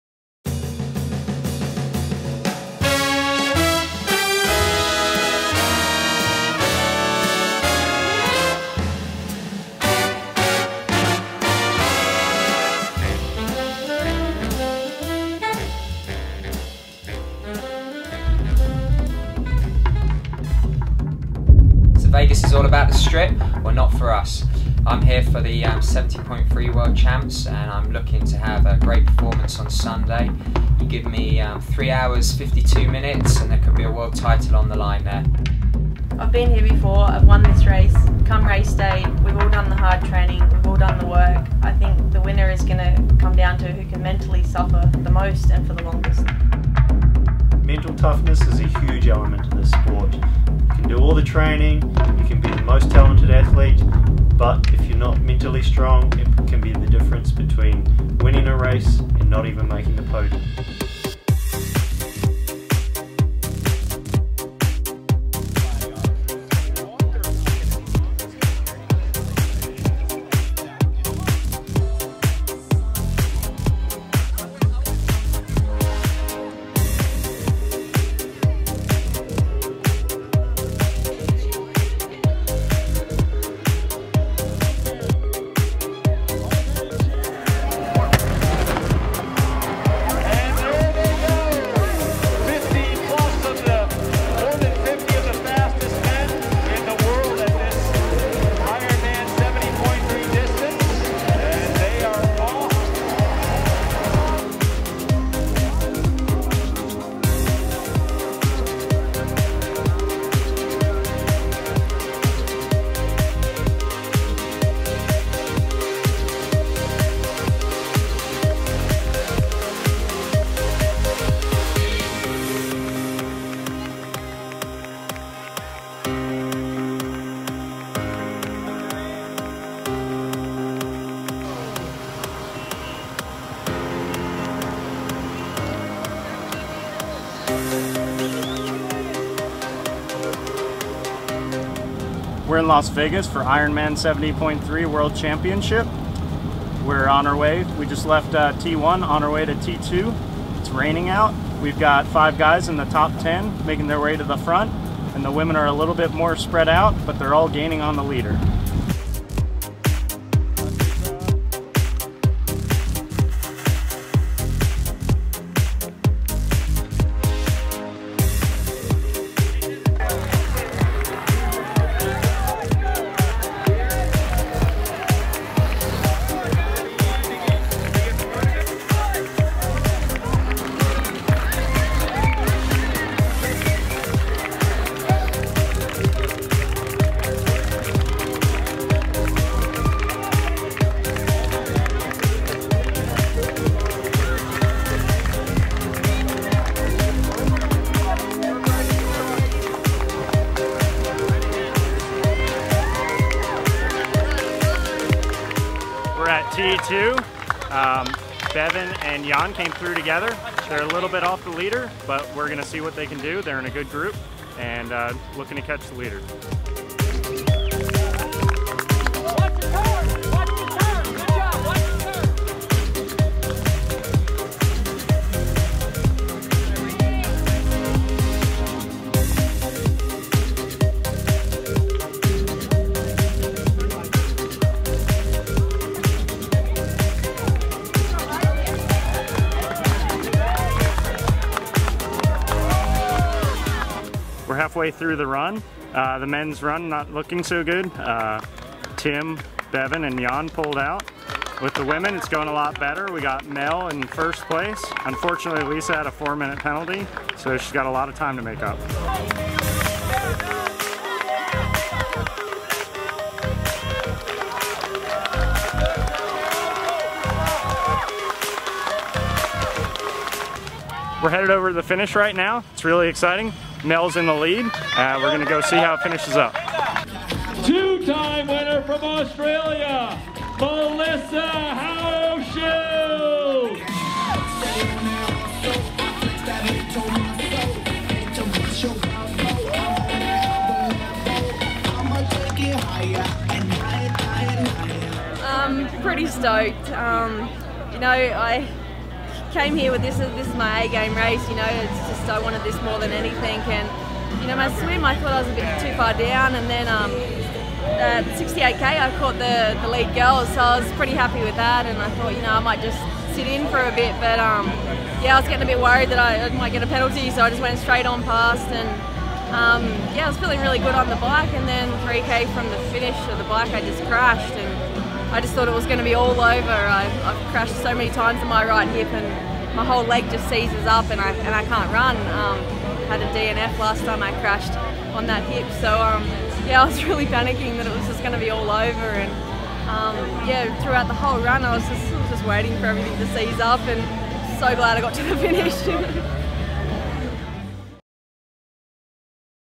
Specialized Triathlon - 2013 Ironman 70.3 World Championships
Las Vegas provided the backdrop for the 2013 Ironman 70.3 World Championships, where Specialized triathletes gathered to continue their remarkable season. While the men may not have had the best day, Melissa "Mel" Hauschildt extended her unbeaten streak.

Join the conversation about AEROISEVERYTHING.

Video shot and produced by Specialized Bicycle Components, Inc.